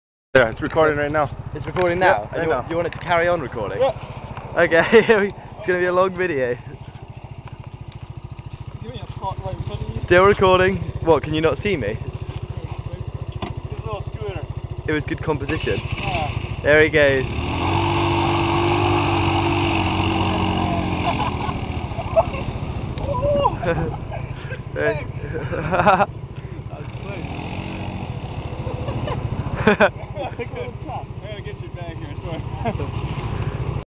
Thailand Scooter Burn Out
Showing these European boys how to do a proper burn out on a bike!